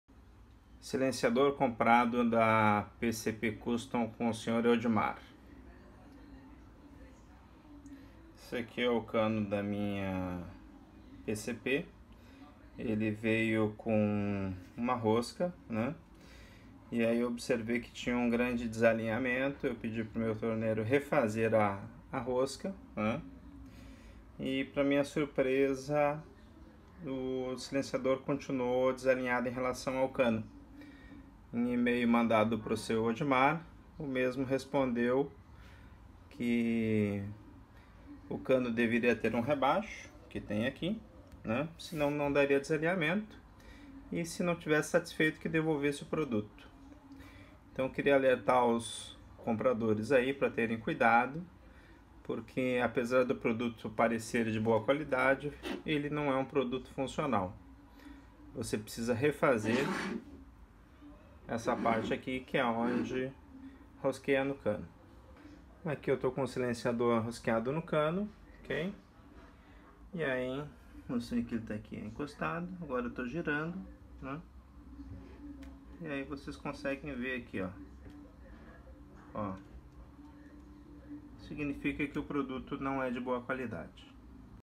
Pcpcuston - silenciador comprado na pcp custon
Pcpcuston - silenciador comprado na pcp custon Com defeito de fabricacao, porem o Sr. Odimar insiste em dizer que seu produto esta perfeito, apenas um alerta para outros futuros compradores, também comprei um kit de PCP com varios problemas, aonde serão corrigidos e posteriormente postados para conhecimento dos leigos. infelizmente o vendedor apenas retornou dizendo que se não estiver satisfeito que devolva o produto...